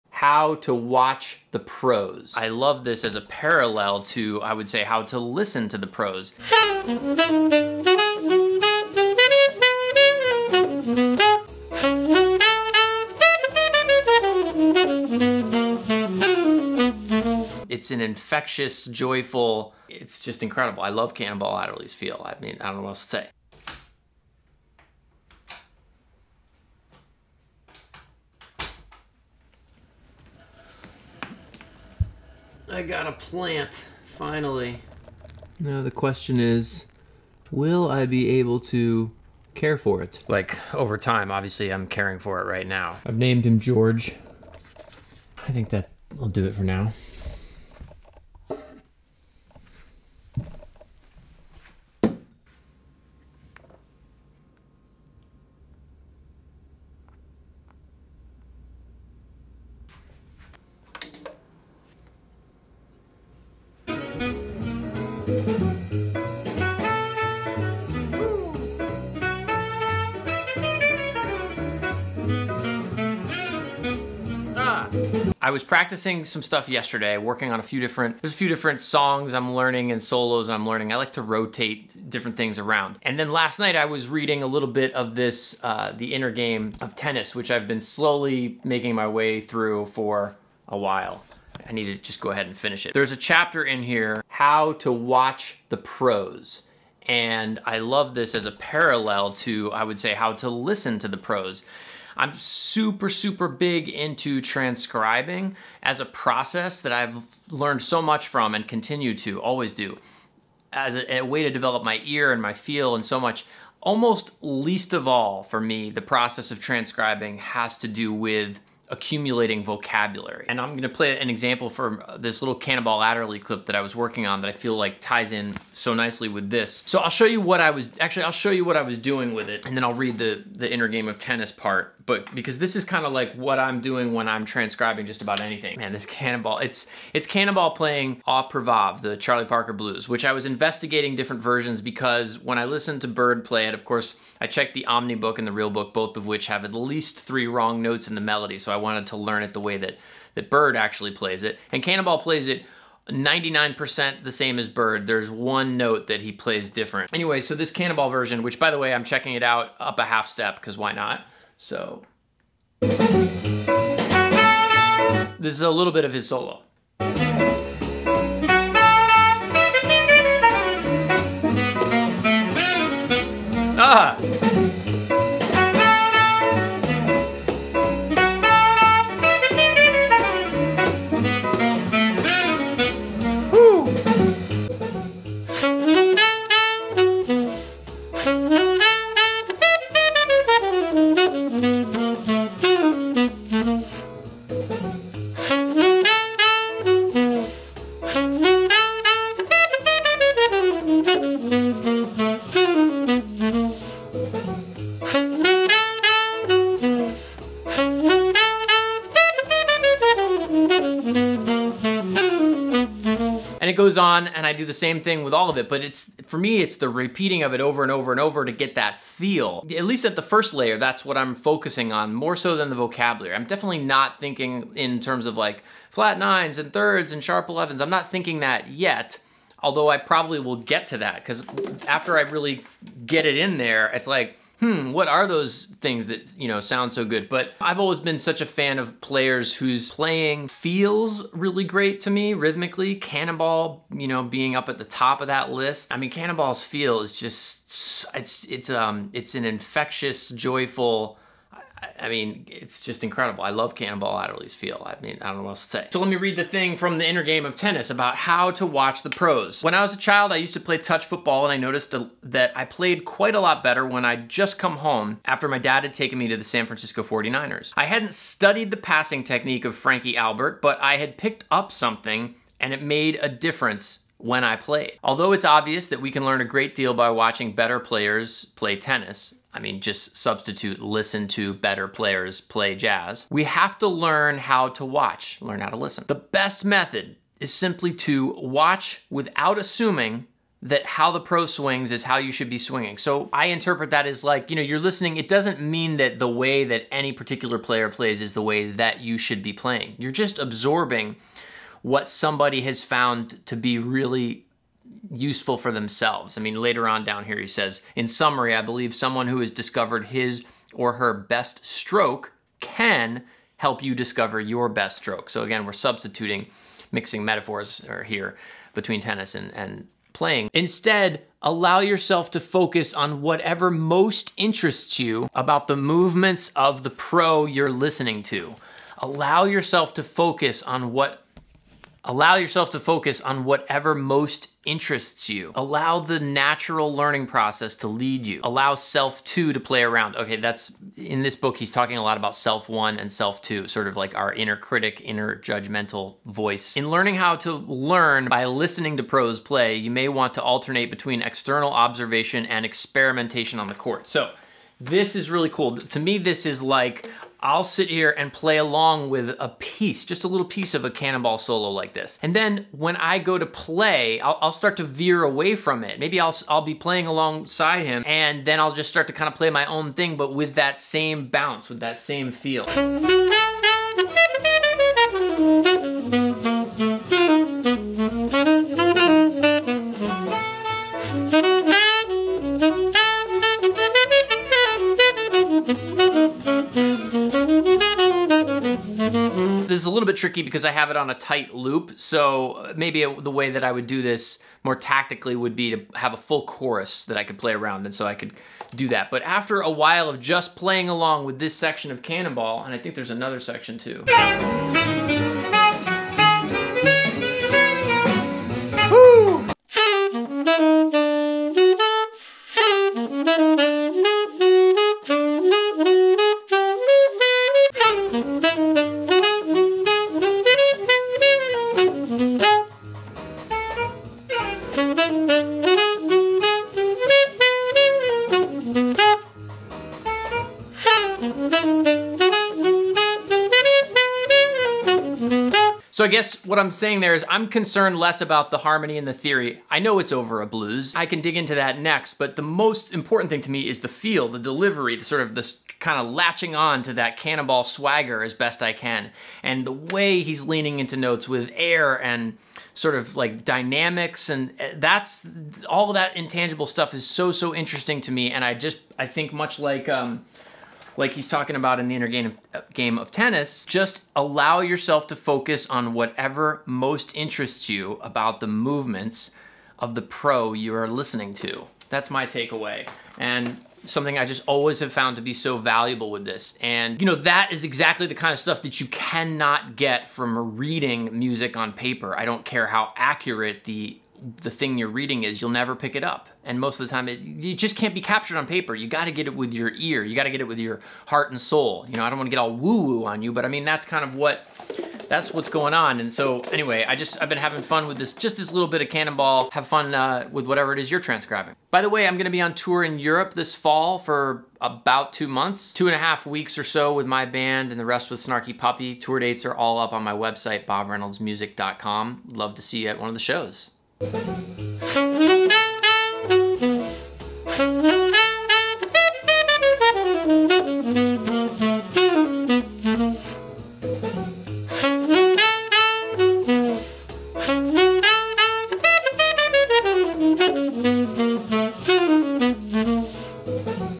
The Art of Listening to Jazz (for Musicians Aiming to Improve)
- A one-page map to creatively organize your practice time.

While reading the book The Inner Game of Tennis, I was struck by a parallel between the author's recommendation for studying tennis pros, and how I've always approached *transcribing* jazz musicians.